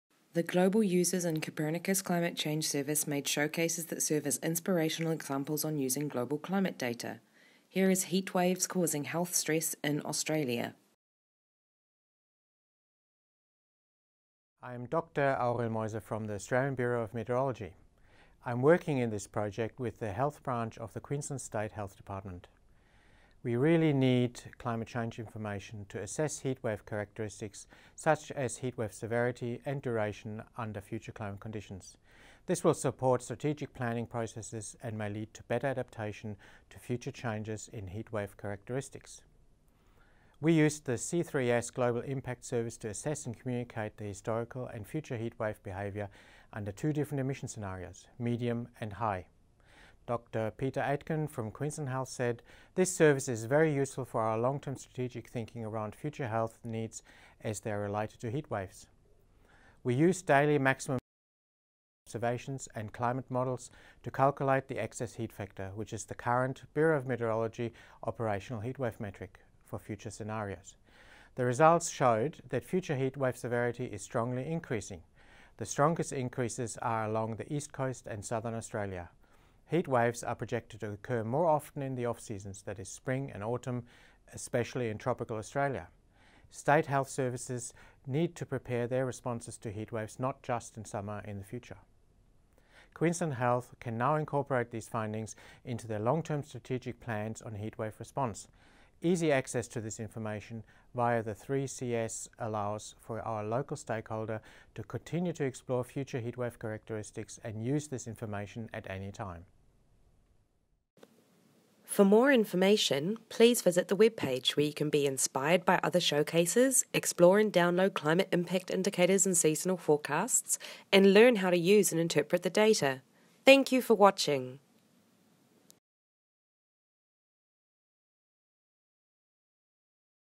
Heat waves causing health stress in Australia
A showcase from BoM, Australia, for C3S_422_Lot1_SMHI contract, as part of Copernicus Climate Change Services (C3S).

to be inspired by other showcases, explore climate impact indicators and seasonal forecasts, and to learn how to interpret climate data.